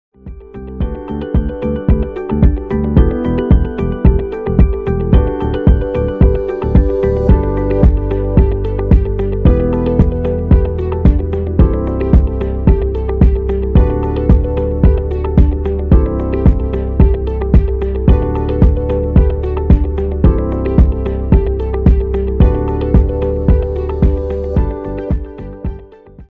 Next is Now 2017 : Fashion Tech Conference
Next Is Now is a 2 day conference offering an immersive experience covering end-to-end tech innovations for the Apparel, Footwear and Fashion Accessories industry.

20 years in the making, Visual 2000 presents the next generation of software. Explore industry trending topics with keynote speakers and training.

Taking place at PHI Center in Montreal, QC, Canada march 29th and 30th.